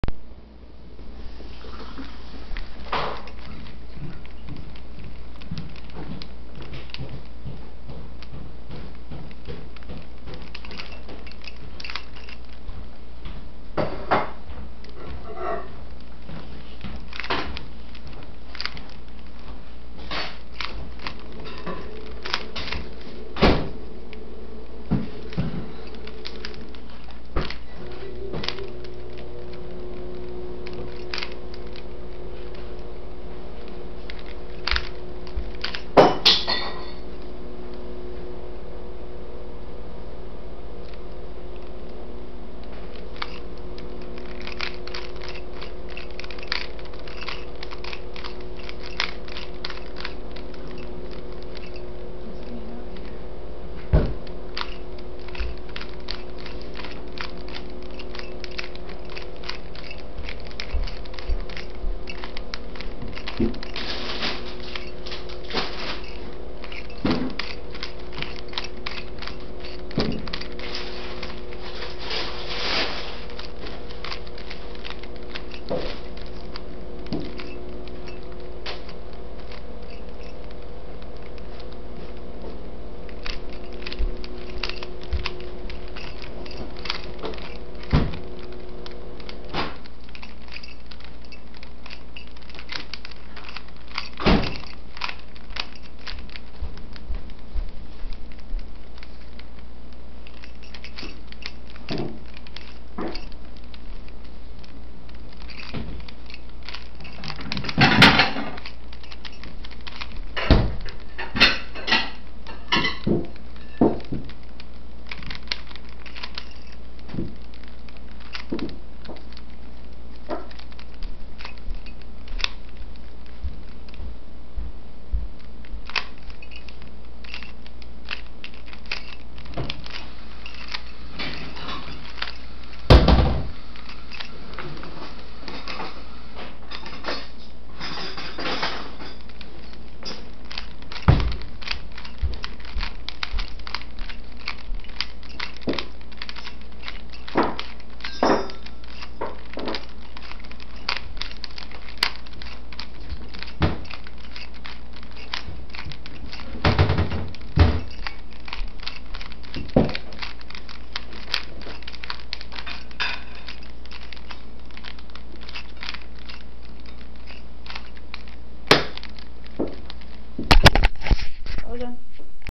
I solve a Rubix Cube! :D
Me, solving a Rubix cube in 2:48 seconds with my awesome Rubix skillz.